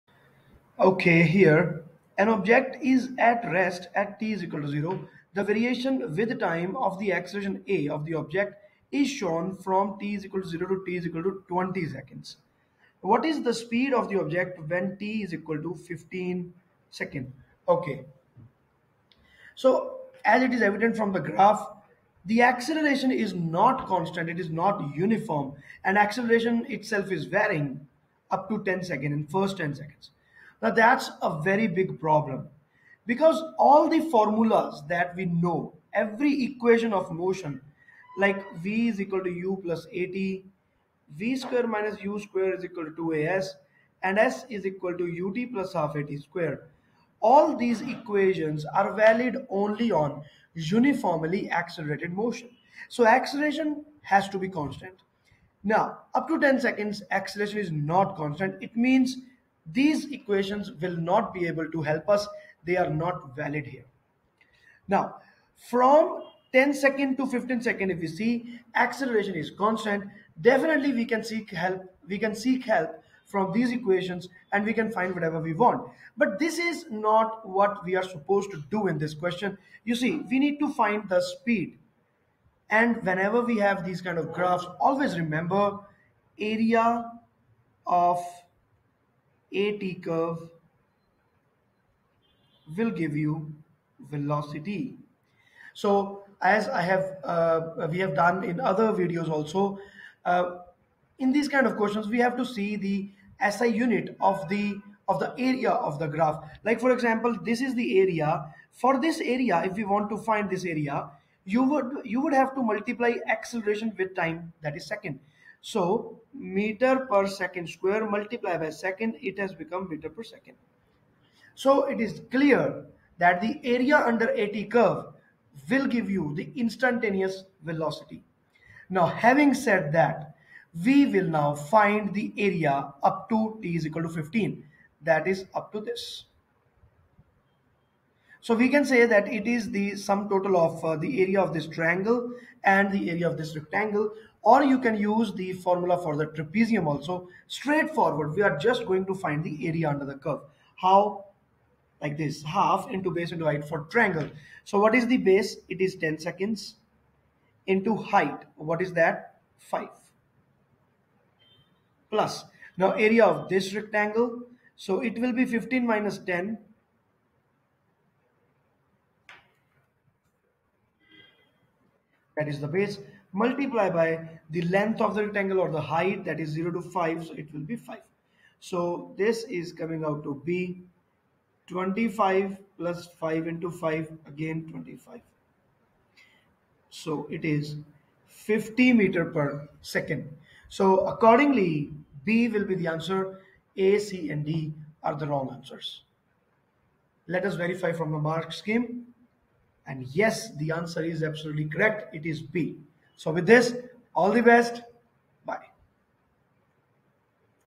An object is at rest at time t=0. The variation with t of the acceleration a of the object is shown
An object is at rest at time t=0. The variation with t of the acceleration a of the object is shown from t=0 to t=20 s. What is the speed of the object when t=15 s?

Valid forever.!